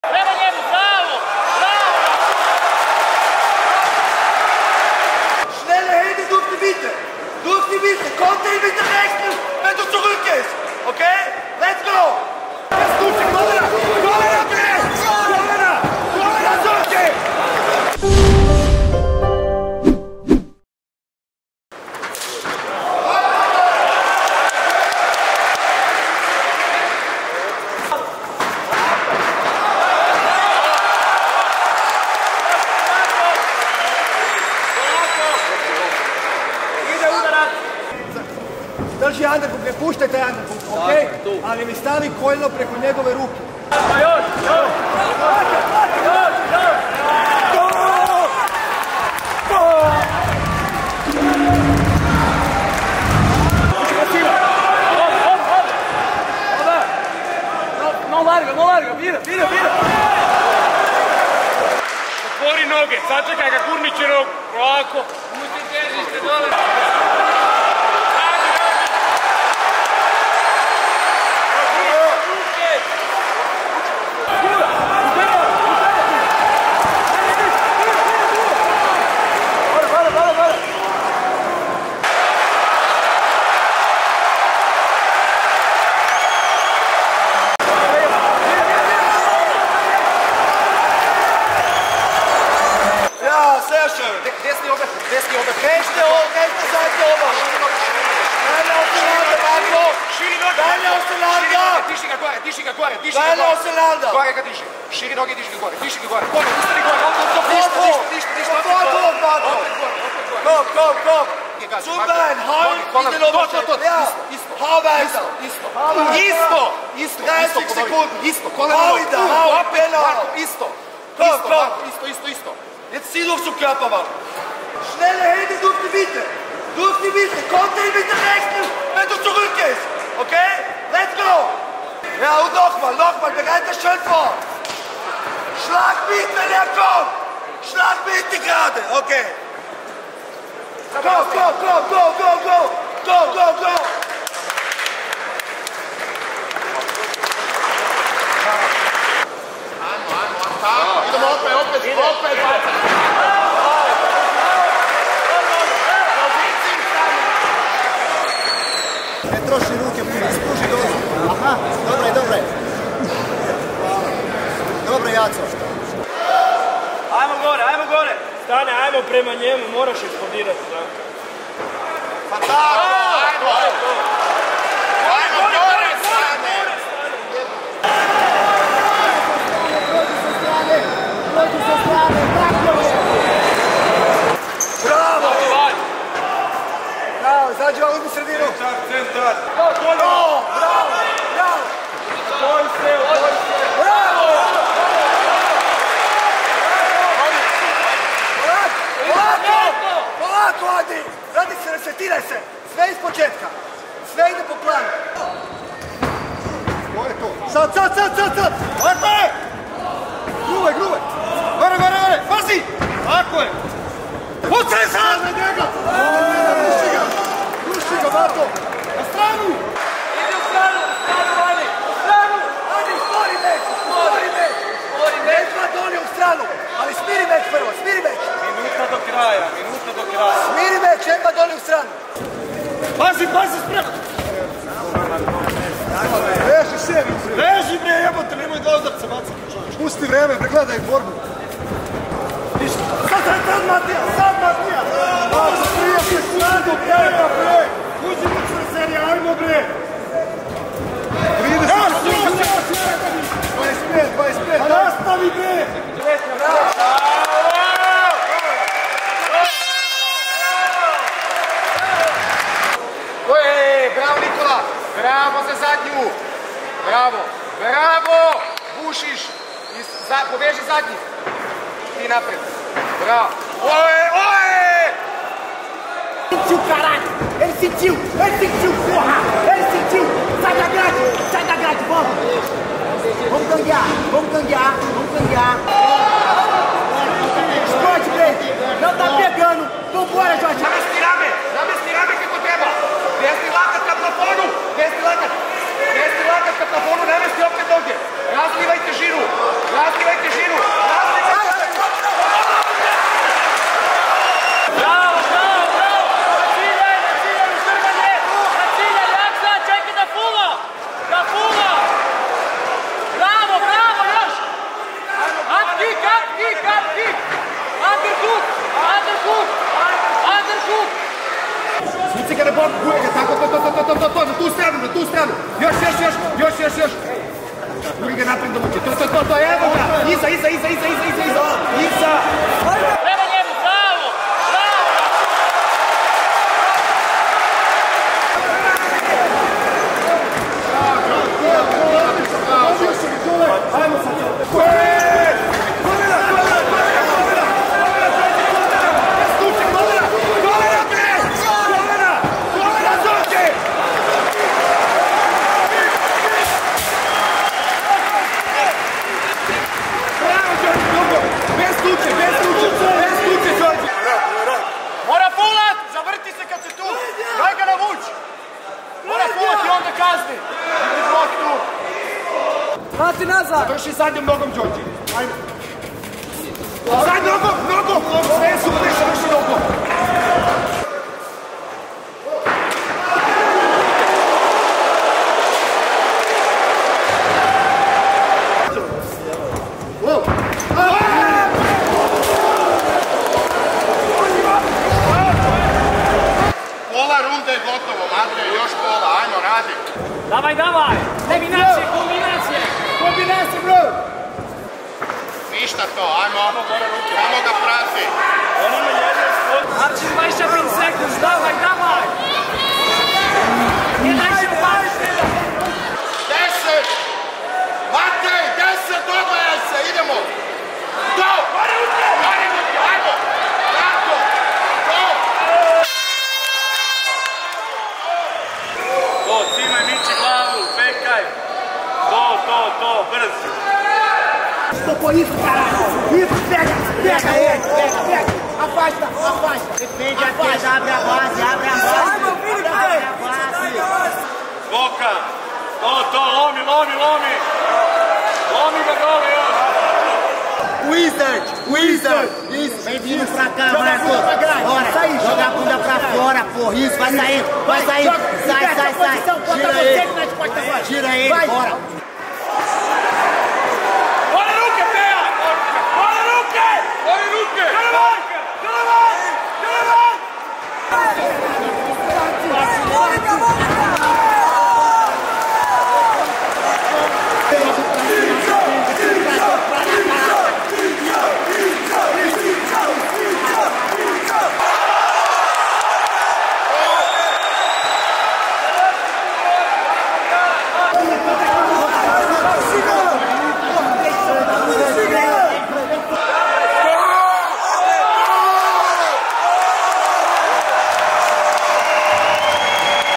Slavlje u kutu Pejića, Bukvića i Vrtačića, razočaranje kod protivnika | CORNER CAM | FNC 9
Napeto je bilo u kavezu, ali napeto je bilo i izvan njega. Pogledajte reakcije na velike pobjede tima iz ATT Zagreb, Trojana i Unity Fight-a.

Glavni urednik 24sata: Ivan Buča